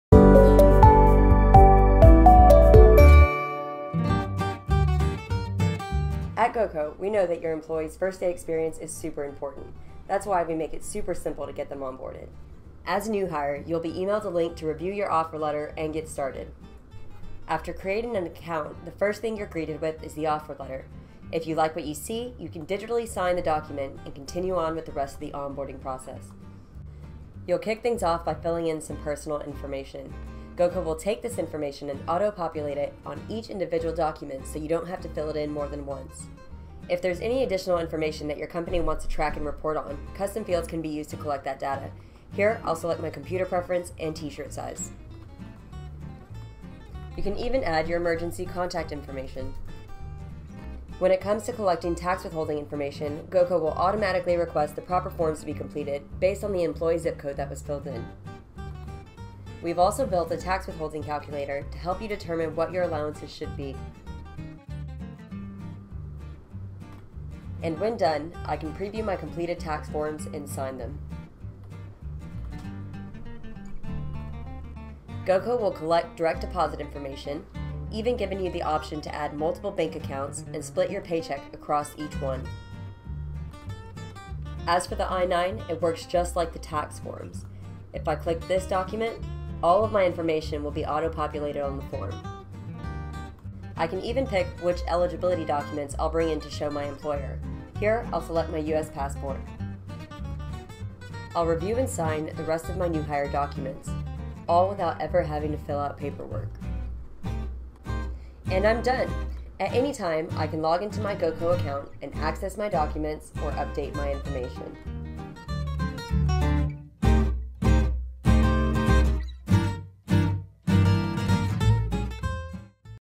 Onboarding an Employee in 2 minutes - GoCo HR Software
- Onboarding a new employee should be simple for HR and a great experience for the employee. With GoCo, both HR and a new employee can be onboarded in just minutes.